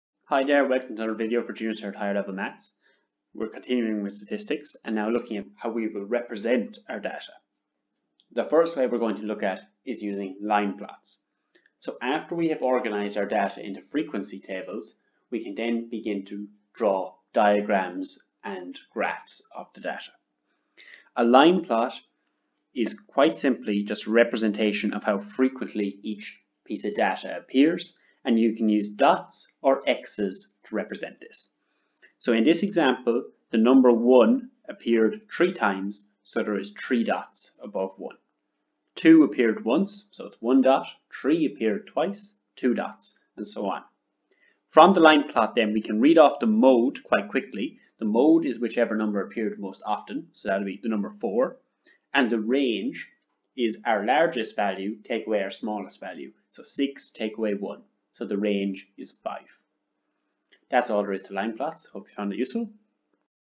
Line Plots - Statistics
Displaying data using Line Plots.
Suitable for Junior Cycle Maths students.